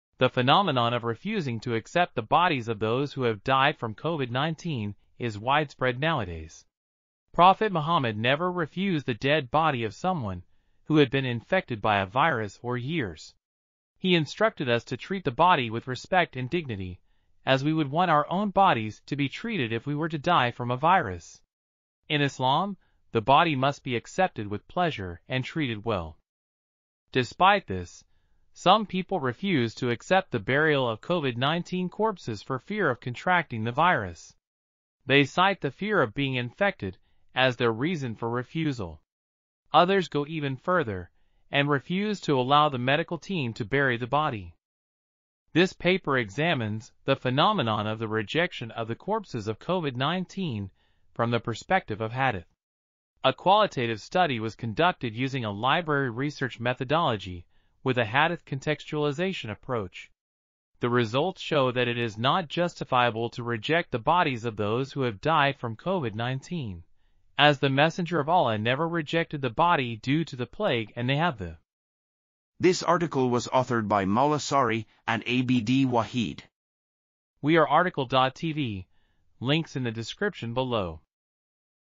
Fenomena Penolakan Jenazah Covid-19 Perspektif Hadis di Indonesia | RTCL.TV
### Keywords ###

### Article Attribution ###
Title: Fenomena Penolakan Jenazah Covid-19 Perspektif Hadis di Indonesia
Authors: Maula Sari ,and Abd Wahid
Publisher: Pascasarjana Universitas Islam Negeri Imam Bonjol Padang
DOI: 10.15548/mashdar.v2i1.1352

### Video Timestamps ###
0:00:00 - Summary
0:01:12 - Title
0:01:17 - Outro
0:01:21 - End